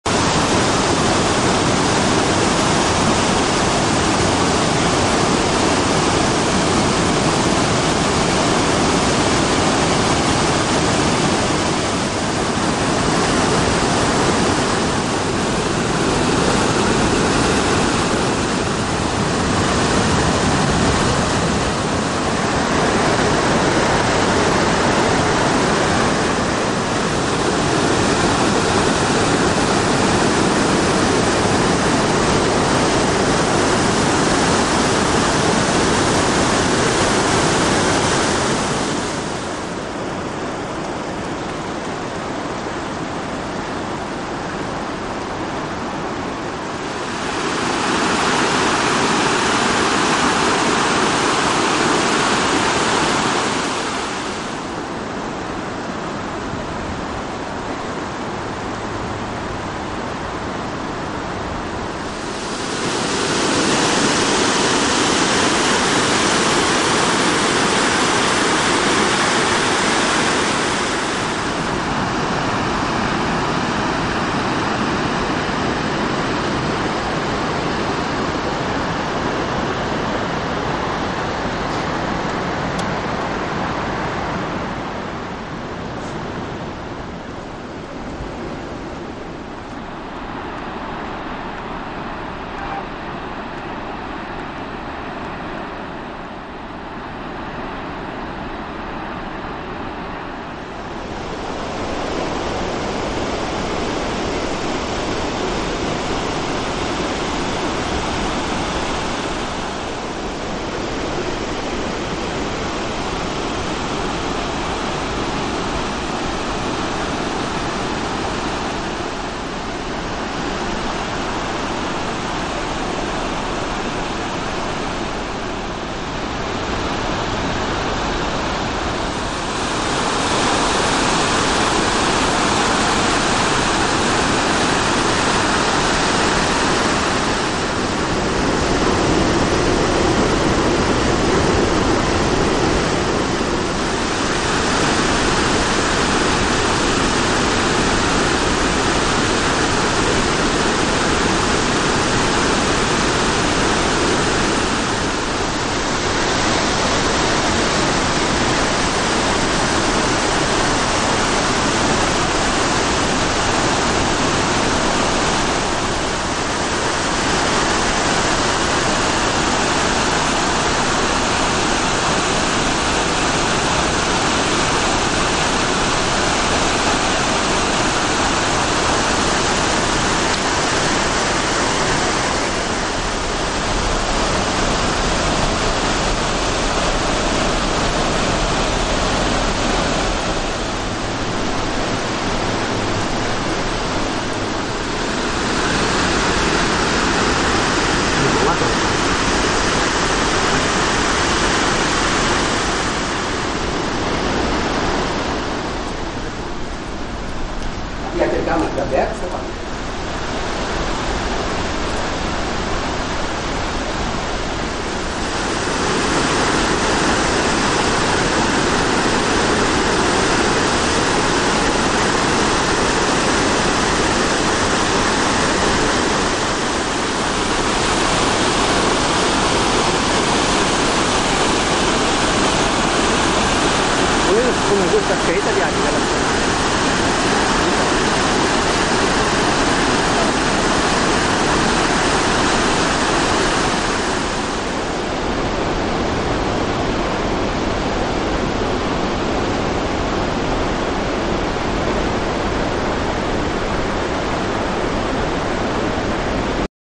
6-11-2010...situazione fiume brenta, e la diga del corlo.....avi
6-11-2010. sono tornato allo sbarramento di campolongo, per vedere la situazione...poi mi sono spostato a cismon del grappa dove passa il torrente cismon...e da li sono salito in località incino dove si trova la diga del corlo, uno degli invasi sul torrente cismon...tra la provincia di vicenza e belluno.